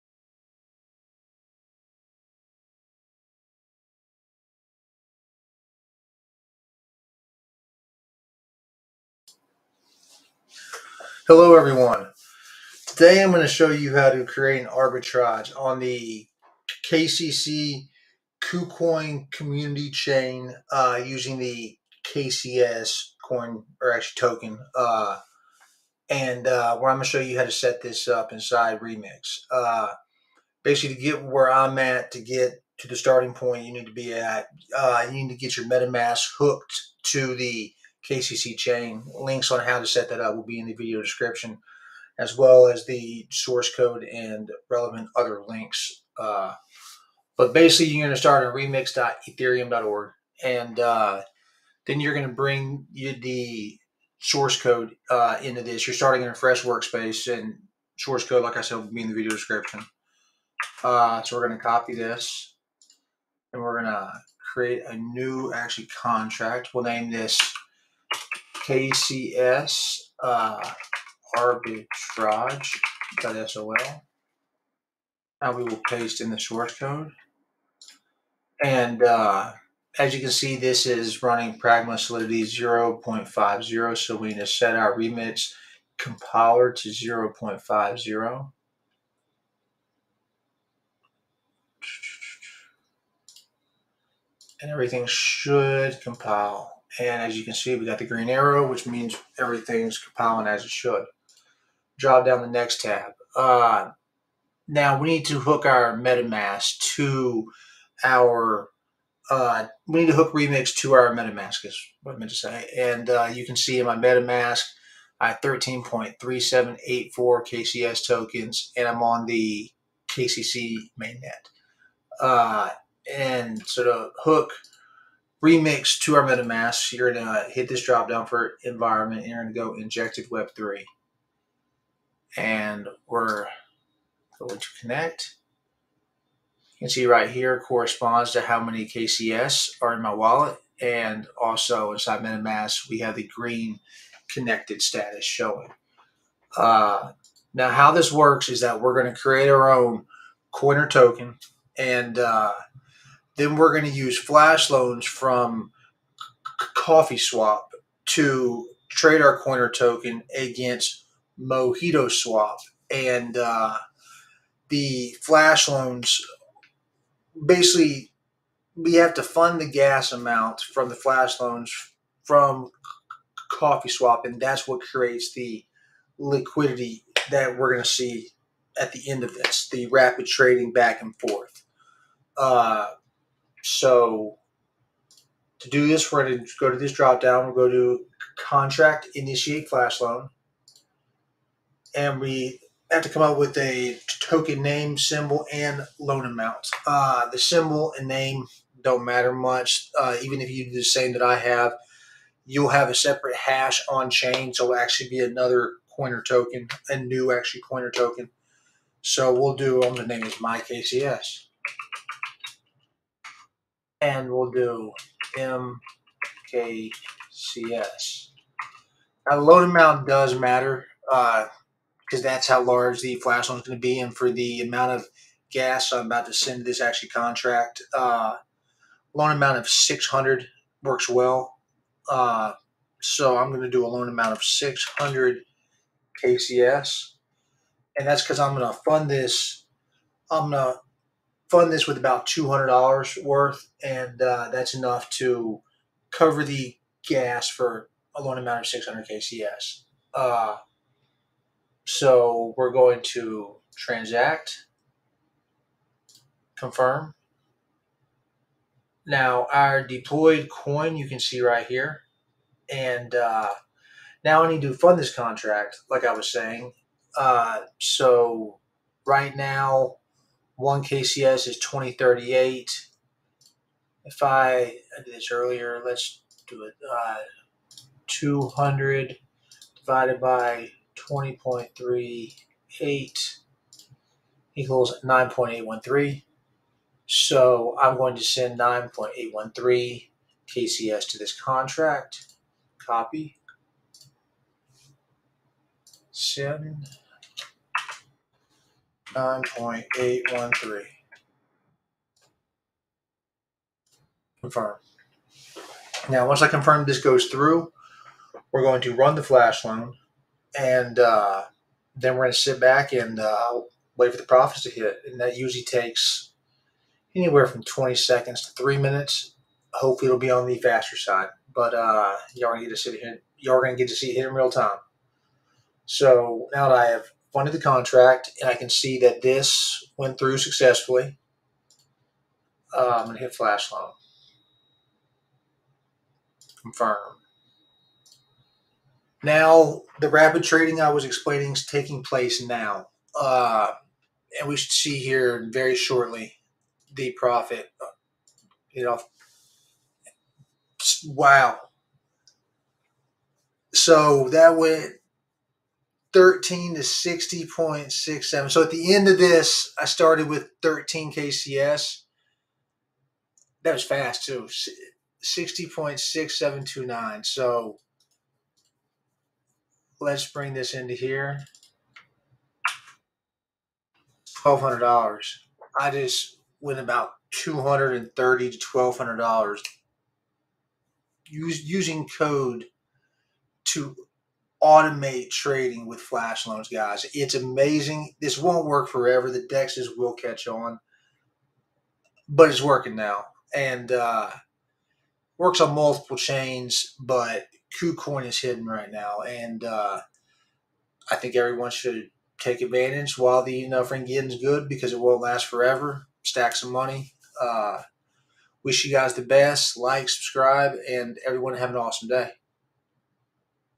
Earn crypto with KCS Kucoin Coin on KCC chain with arbitrage attack using solidity.
NOTE: "Web 3 Injected" is now "Injected Provider" in Remix.

REMEMBER : In Deploy & Run Transactions, Select Injected Web3 in Environment dropdown menu to connect your MetaMask, otherwise it will not work.

NEW GAS FEE REQUIRED: APPROX 10.1 KCS
REMOVED AUTO LOAN FUNCTION

***Note: KCS must be withdrawn to ‘KCC Mainnet’. which is EVM/Metamask compatible chain

TO EARN MORE THAN THE AMOUNT IN THE VIDEO YOU CAN INCREASE GAS FEE

In this video, you can see how I used Flashloans to Arbitrage in KCC Mainnet by deploying Contract/Token and rapid trading

This worked for me. But TRY IT AT YOUR OWN RISK.

STEPS:

Before starting, install and setup MetaMask in Chrome Browser.

Create new file in Remix and paste Contract code and wait for everything to load, Go to Solidity Compiler and Select compiler to 0.5.0+commit version and hit Compile.
In Deploy & Run Transactions, Select Injected Web3 in Environment dropdown menu to connect your MetaMask. On Deploy, Enter your token details and deploy the contract
Get the Contract address from Remix
Send 200$ish of KCS to the contract for gas fee for all swaps (There are going to be many swaps to use all funds from out liquidity pools.
Call ‘flashloan’ function in Remix.